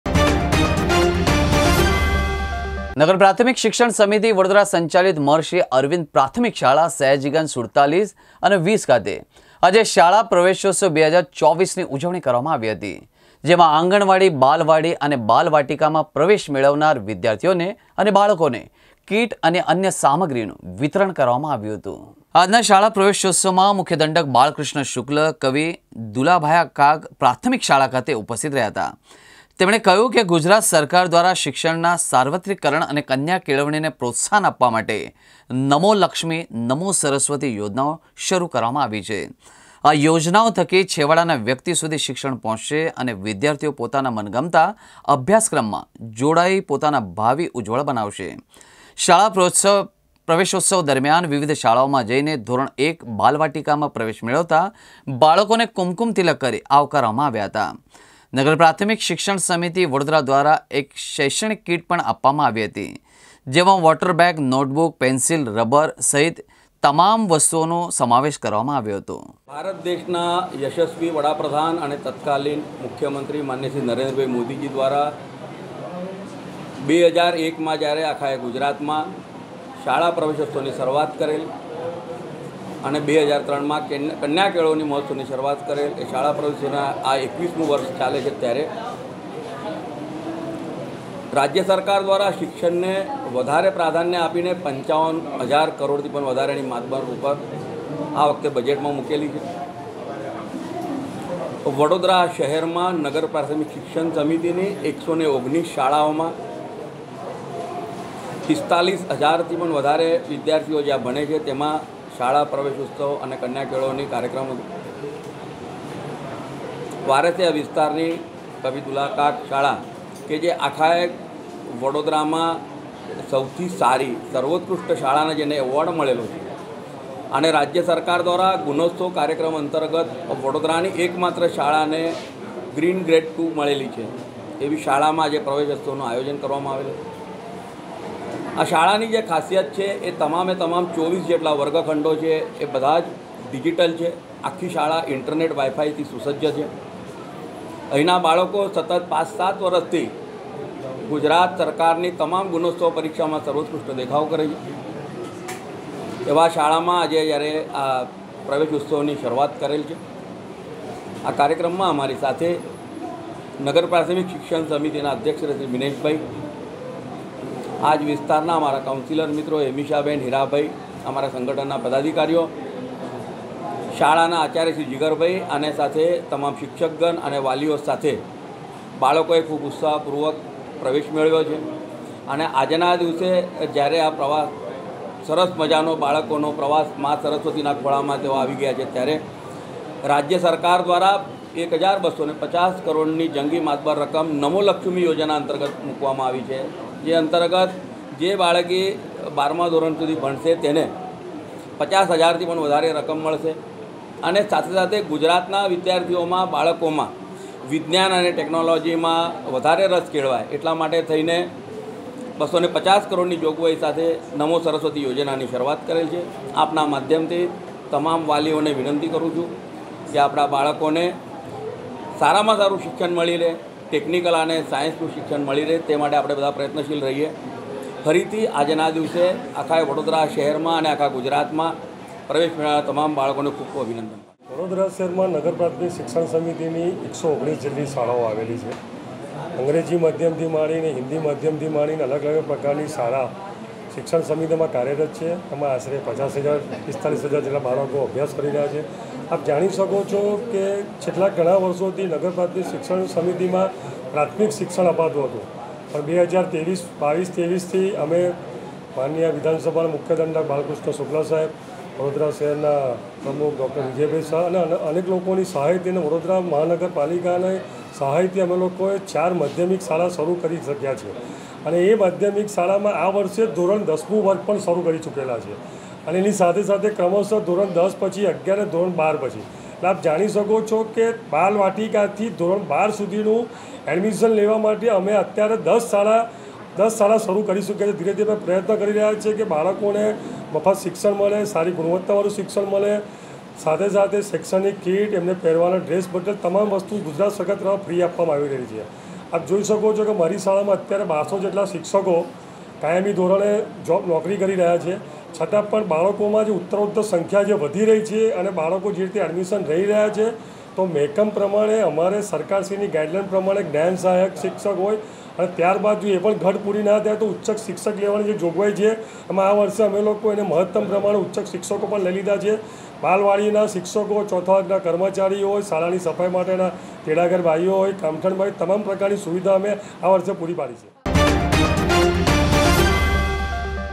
શાળા પ્રવેશોત્સવ-૨૦૨૪ નો ભવ્ય શુભારંભ | G VAN News
નગર પ્રાથમિક શિક્ષણ સમિતિ વડોદરા સંચાલિત મહર્ષિ અરવિંદ પ્રાથમિક શાળા, સયાજીગંજ 47 અને 20 ખાતે આજે શાળા પ્રવેશોત્સવ 2024 ની ઉજવણી કરવામાં આવી. જેમાં આંગણવાડી , બાલવાડી અને બાલવાટિકામાં પ્રવેશ મેળવનાર વિદ્યાર્થીઓને  બાળકોને કીટ અને અન્ય સામગ્રીનું વિતરણ કરાયું